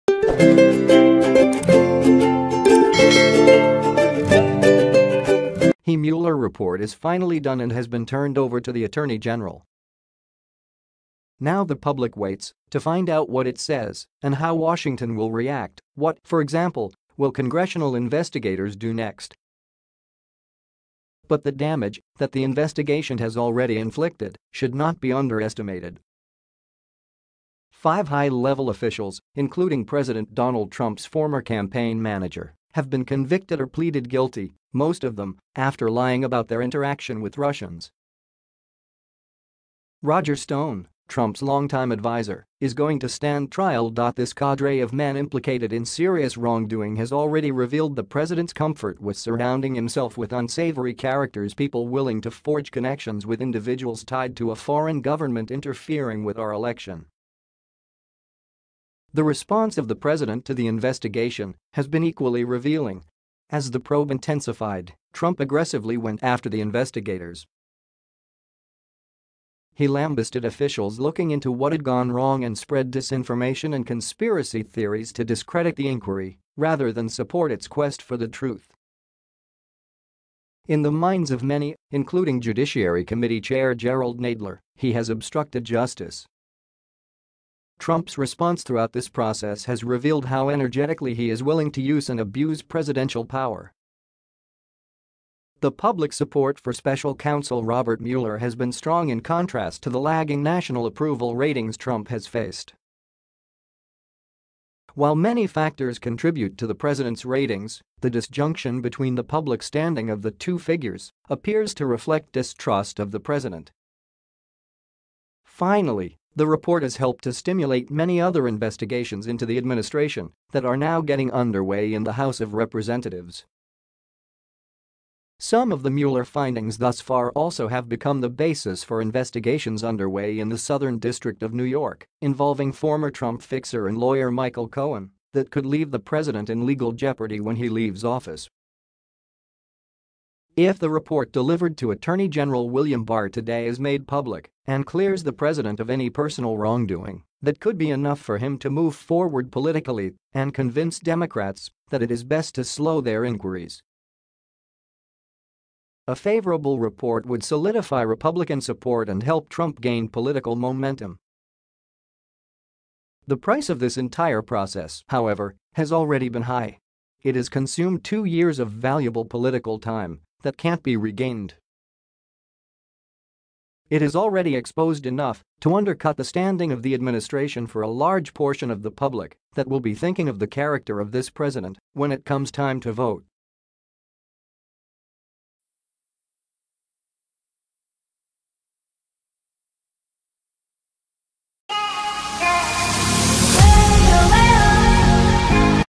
Mueller's probe has already revealed something about Trump
he Mueller report is finally done and has been turned over to the attorney general.  Now the public waits to find out what it says and how Washington will react (what, for example, will Congressional